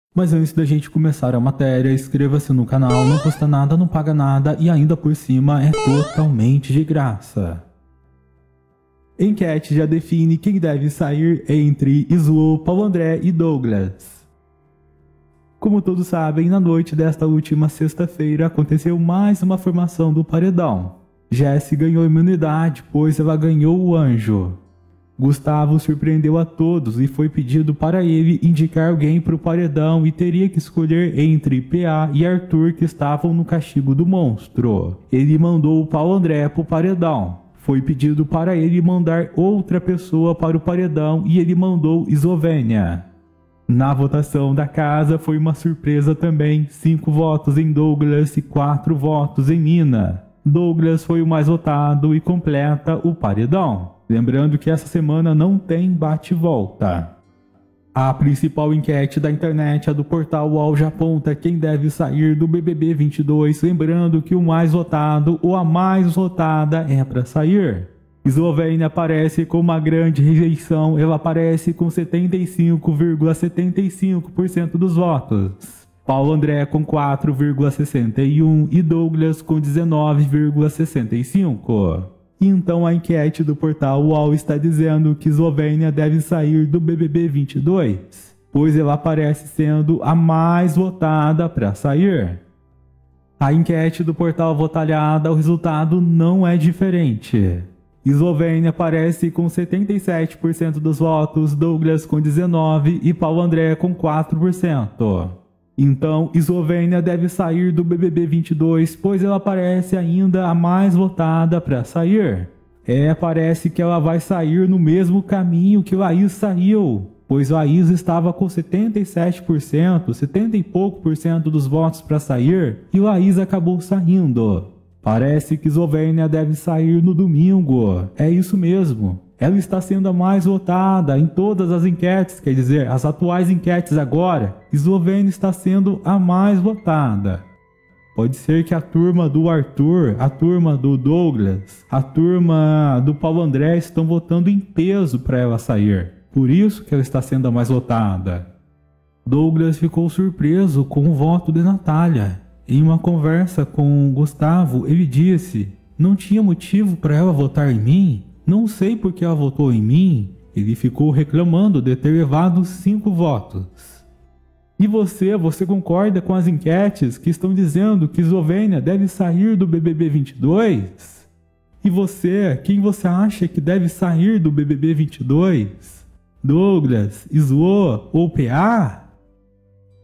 🔴 BBB22: Enquete Atualizada, Já diz Quem deve sair entre Paulo André, Eslô e Douglas
Atuais participantes do BBB22
Arthur Aguiar (Arthur), Lucas, Paulo André (PA), Douglas Silva (DG), Pedro Scooby (Scooby), Eslovênia (Eslô) Natália, Jessilane (Jessi), Linn da Quebrada (Lina), Gustavo, Eliezer (Eli).